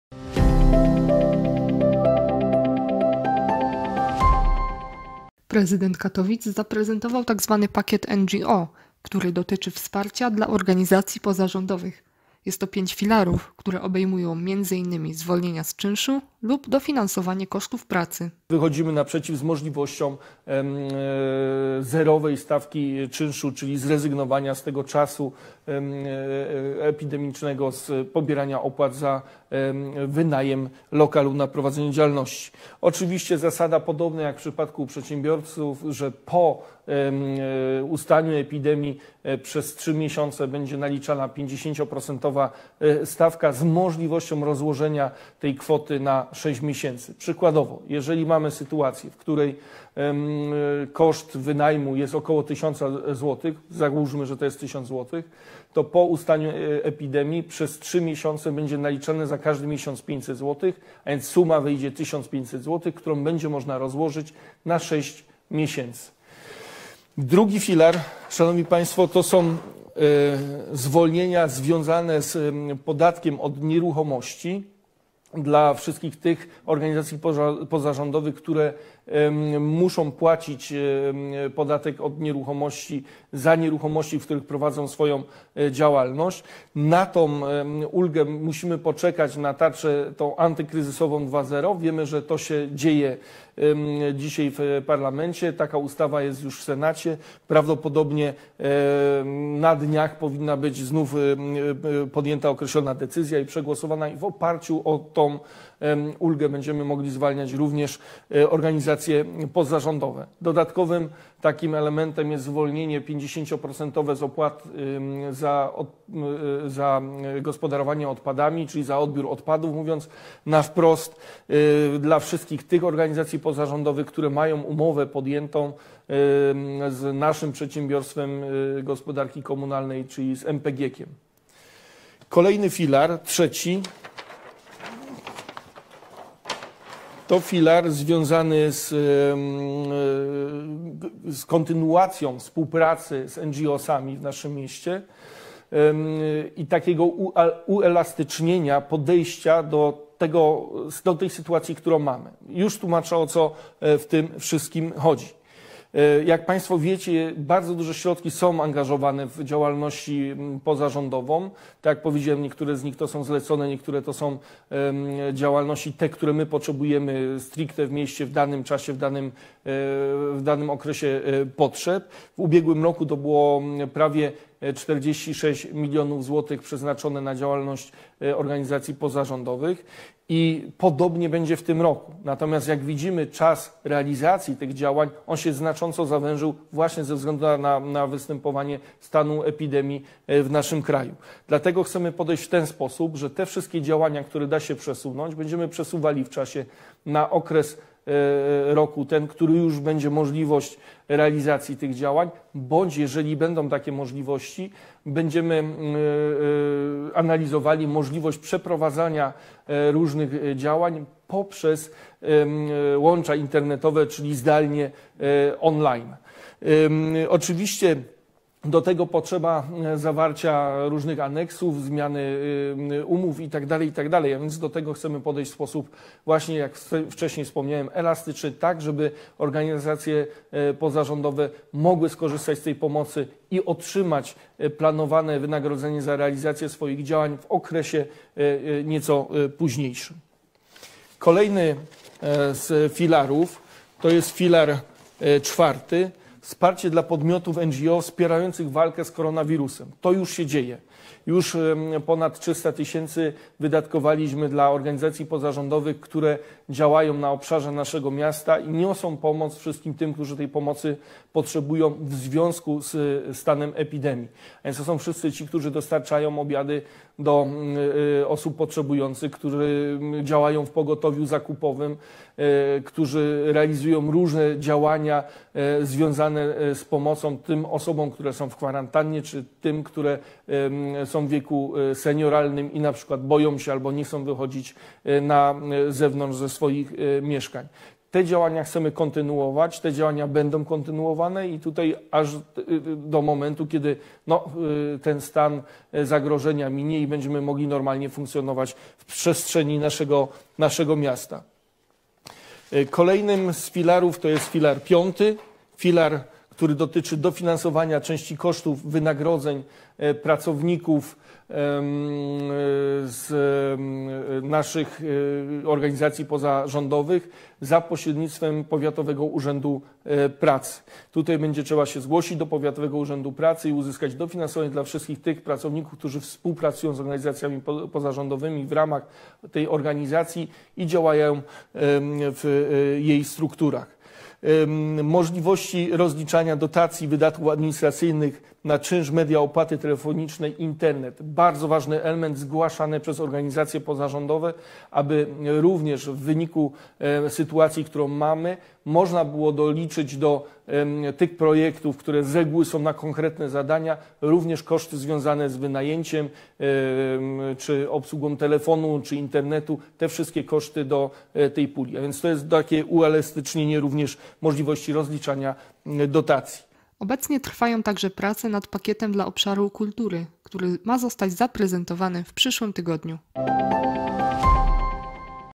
Katowicki Pakiet NGO
W Katowicach sektor organizacji pozarządowych dynamicznie się rozwija i liczy dziś ponad 1650 podmiotów. Organizacje NGO realizują zarówno zadania zlecane i finansowane przez Miasto Katowice, jak i prowadzą własną działalność statutową. Zgodnie z moimi zapowiedziami, prezentujemy Katowicki Pakiet NGO, który zawiera rozwiązania możliwe do wdrożenia przez władze samorządowe w aktualnie obowiązujących ramach prawnych.